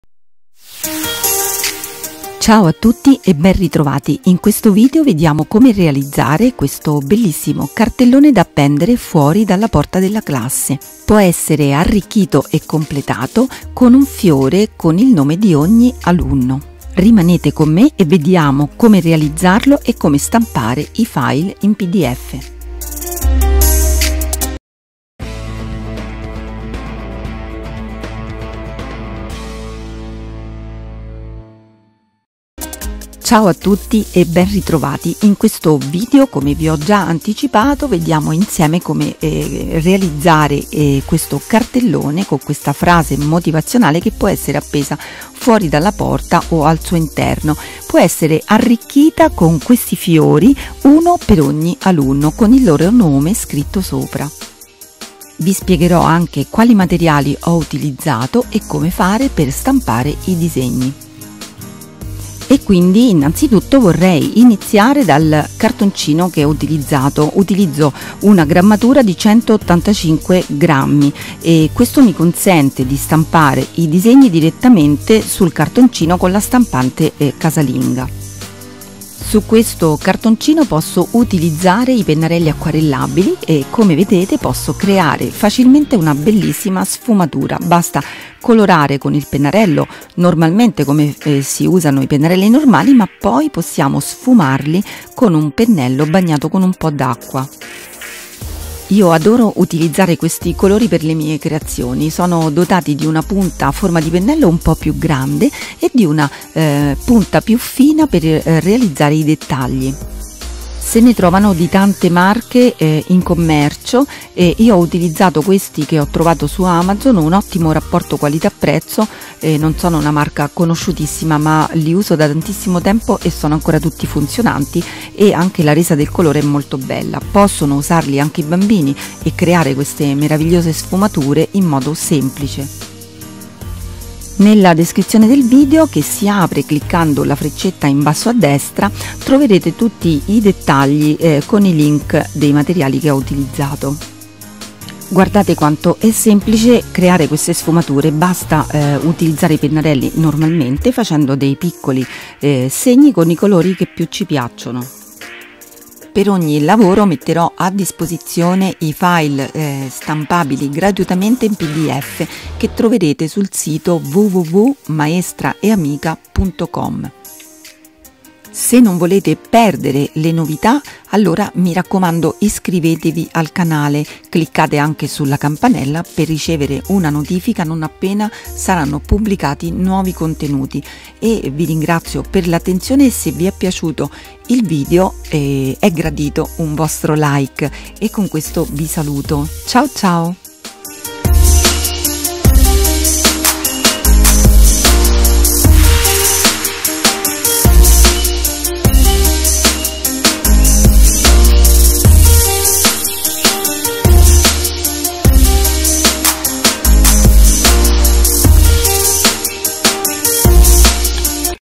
Accoglienza: cartellone (3) fuori porta con frase motivazionale e tag alunni. PDF gratuiti
Clicca qui e trovi i file da scaricare in fondo all'articolo

COLLA ATOM FIX
PENNARELLI ACQUARELLABILI BOIROS
PENNELLI CON SERBATOIO
PACCO CARTONCINO CANSON A4 185 g/m2